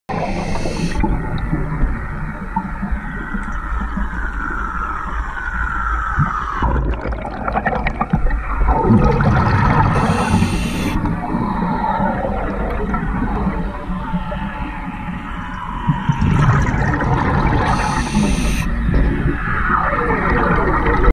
Diving Na Stacain, Kinlochbervie
Inside the cave at Na Stacain, Kinlochbervie. Diving with Atlantic Diving Services on the MFV Lady Nicola.
Find our dive trips on Facebook Highland Divers